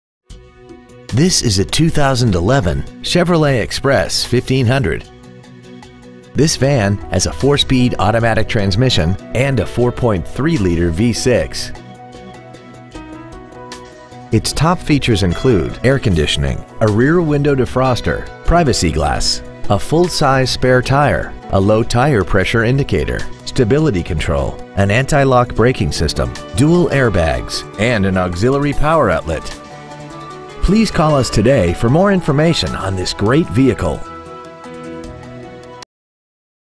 2011 Chevrolet Express Cargo Chandler AZ 85225
Year : 2011
 Make : Chevrolet
 Model : Express Cargo
 Engine : 4.3L V-6 Electronic Fuel Injection
 Trans . : 4 Speed Automatic
 Exterior : Summit White
 Miles : 14

 Stock : T115900

 2121 N. Arizona Avenue